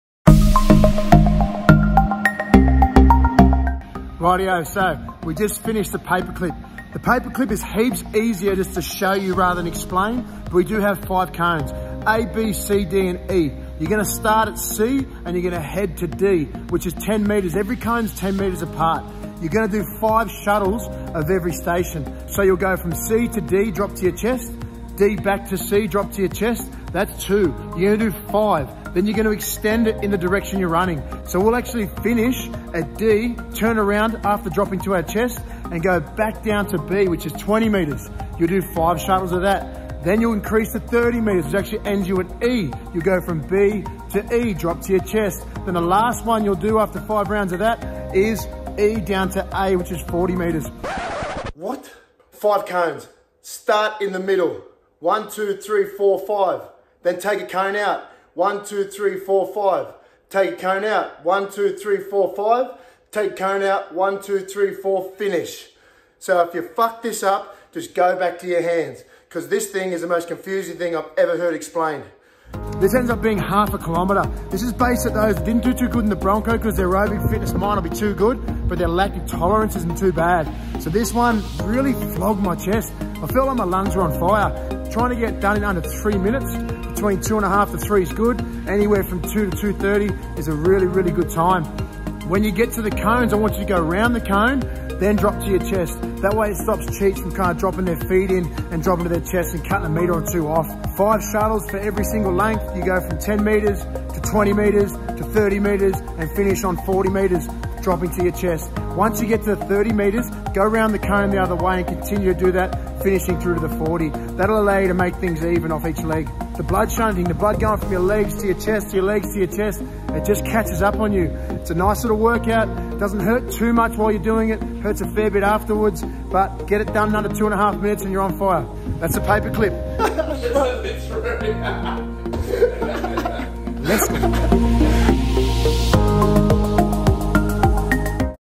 CONDITIONING - The Paperclip Test - COVID-19 - Pinpoint Online
Confusing but effective when learnt! 5 cones, A,B,C,D,E all spaced 10 meters apart. the entire distance will be 40m. Start in the middle at C and run 5 shuttles between C & D, dropping to your chest at each cone. Once you finish the 5 shuttles, remove the start cone from the centre (C) and continue which ends up being 20m and from D to B, 5 shuttles than remove the start cone D and continue B to E (30m) x 5 shuttles than the final set is 40m which is E - A.

Watch the movie for a better explanation. Aim for under 3 minutes. 2:30 is fast! Goodluck!